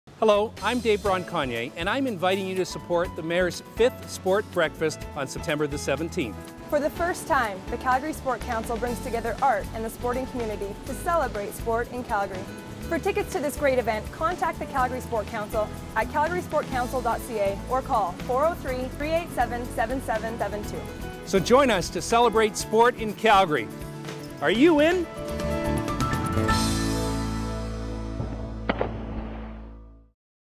Taylor Creative - Mayor's Breakfast
In September 2009 Mayor Dave Bronconnier of Calgary hosted the Mayor's Sport Breakfast. This commercial, fronted by Olympian Regan Lauscher was used to promote the event and we simulated Mayor Dave Bronconnier diving off the 7.5 Metre tower.